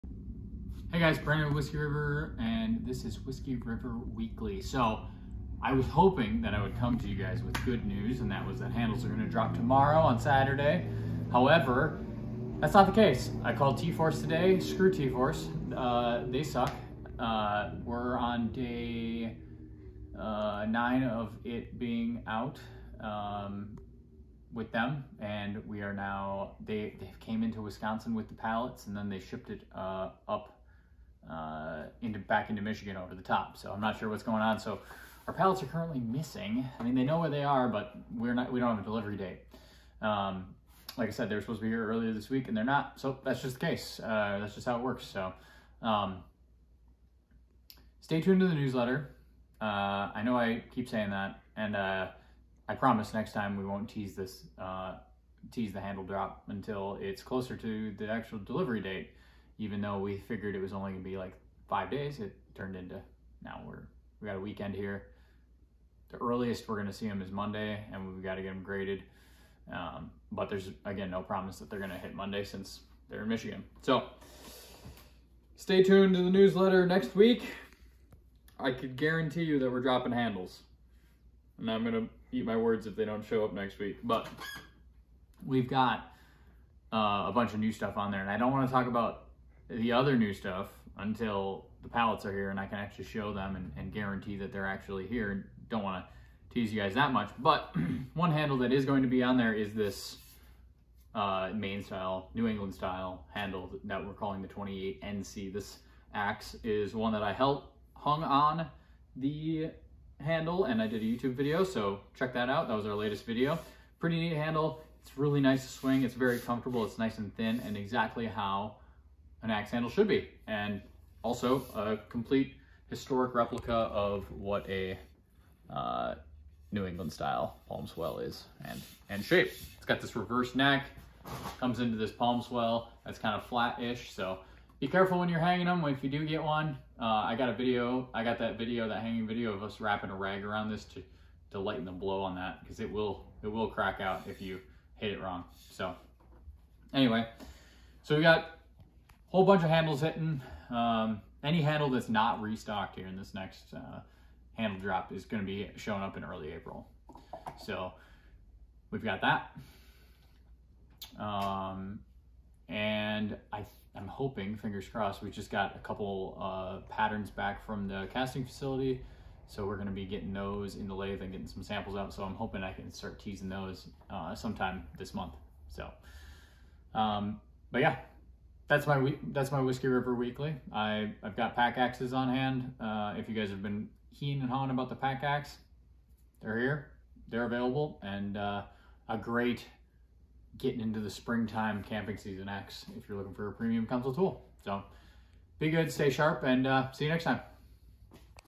Whiskey River Weekly // Vlog Ep. 42
A certain shipping company has left us high and dry, they've sent us up sh!t creek without a handle and I'd like to talk about it...